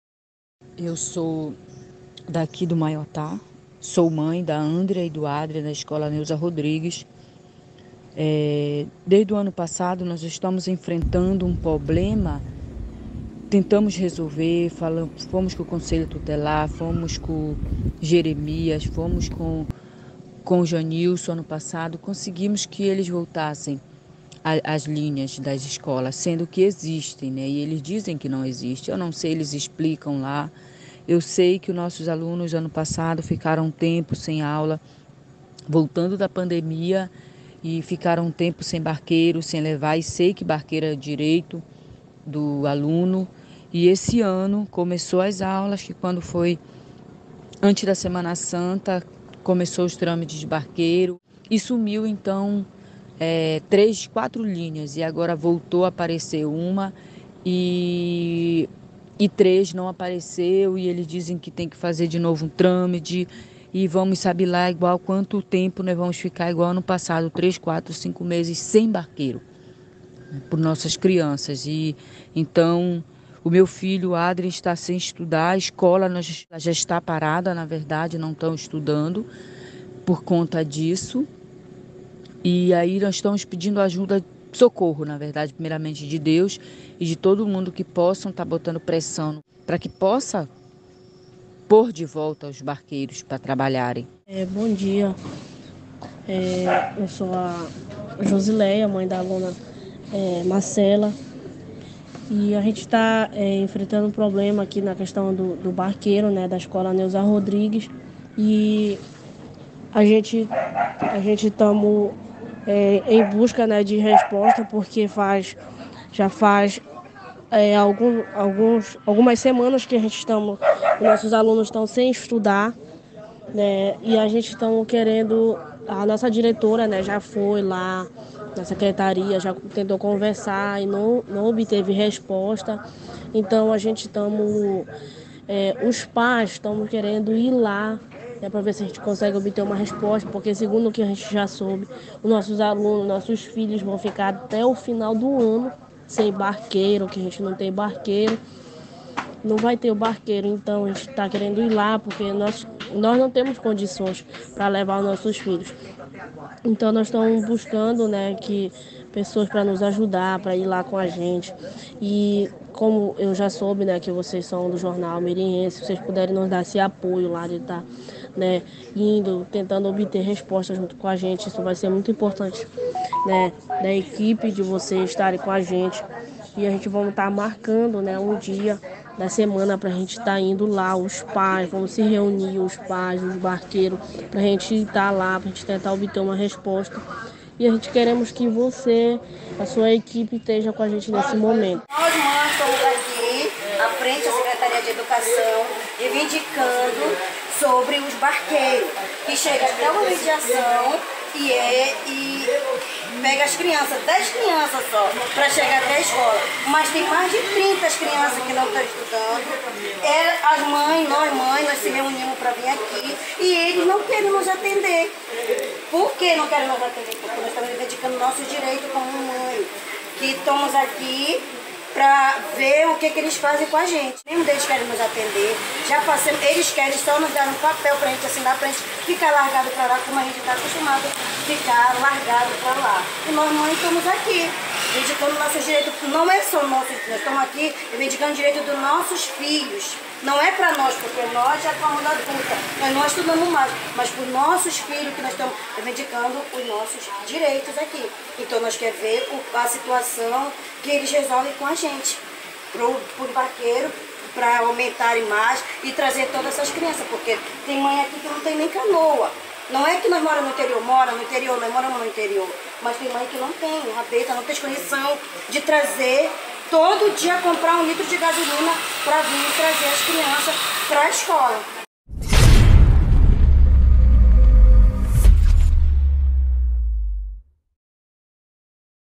FALTA DE TRANSPORTE ESCOLAR, DEIXA ALUNOS DE 2 ESCOLAS RIBEIRINHAS SEM ACESSO A EDUCAÇÃO
FALTA DE TRANSPORTE ESCOLAR, DEIXA ALUNOS DE 2 ESCOLAS RIBEIRINHAS SEM ACESSO A EDUCAÇÃO É O QUE DENÚNCIA MÃES DE ALUNOS DA ESCOLA NEUZA RODRIGUES DA VILA CORREA E DA ESCOLA SÃO SEBASTIÃO DO RIO MINEIRO ZONA RURAL DE IGARAPÉ-MIRI